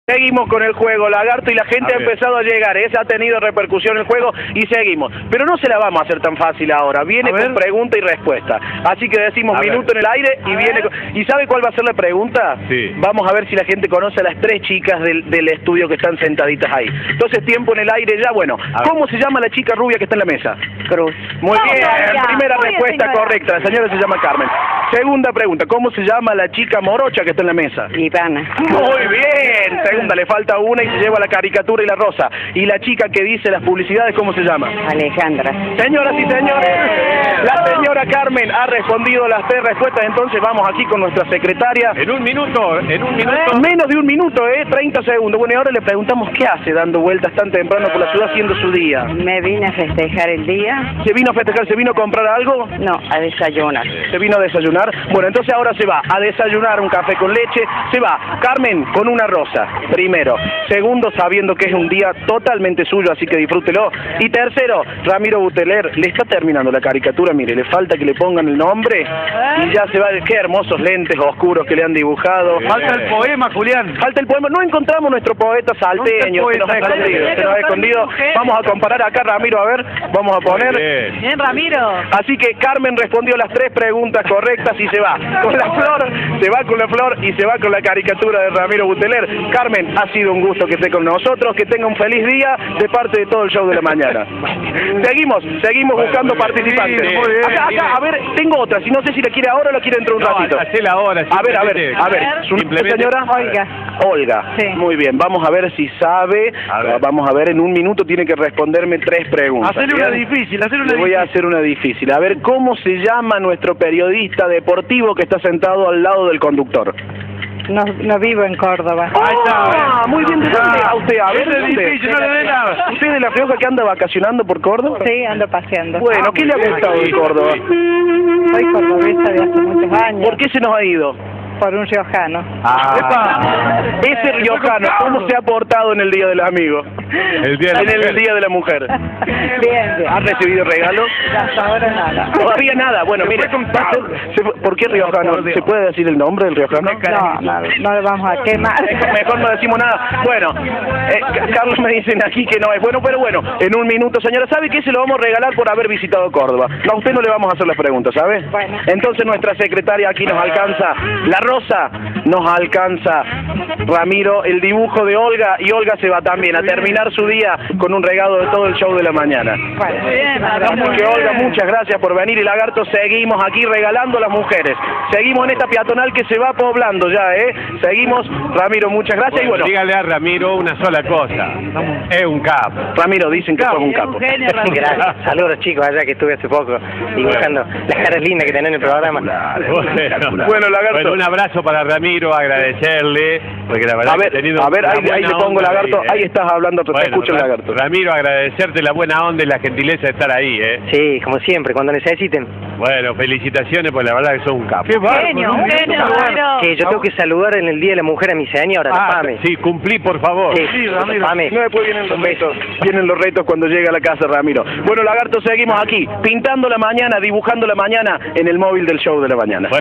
caricaturas flores y poemas en el dia de la mujer pt2 20110308.3gp
Siguiendo con la idea de festejar el Día de la mujer, el caricaturista Ramiro Buteler realiza caricaturas en vivo para las mujeres de la peatonal.